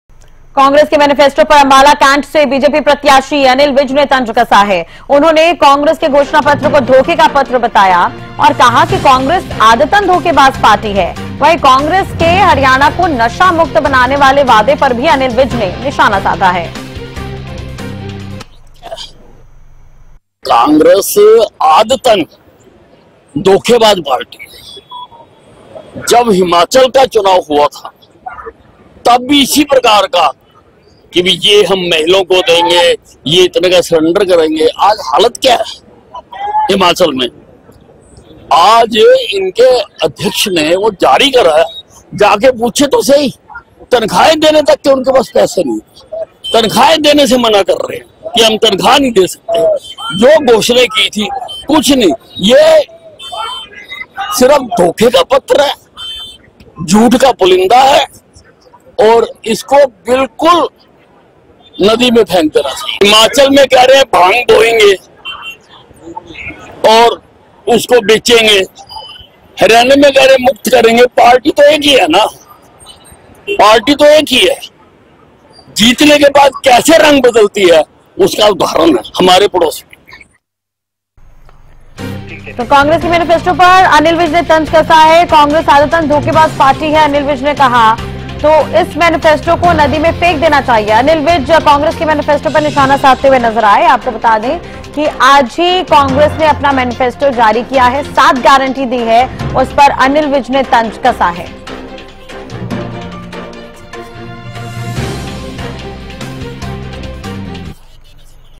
अंबाला: कांग्रेस के मेनिफेस्टो पर अनिल विज का तंज, घोषणा पत्र को बताया धोखे का पत्र
कांग्रेस के मेनिफेस्टो पर अंबाला कैंट से बीजेपी प्रत्याशी अनिल विज ने तंज कसा है. उन्होंने कांग्रेस के घोषणा पत्र को धोखे का पत्र बताया और कहा कि कांग्रेस आदतन धोखेबाज पार्टी है.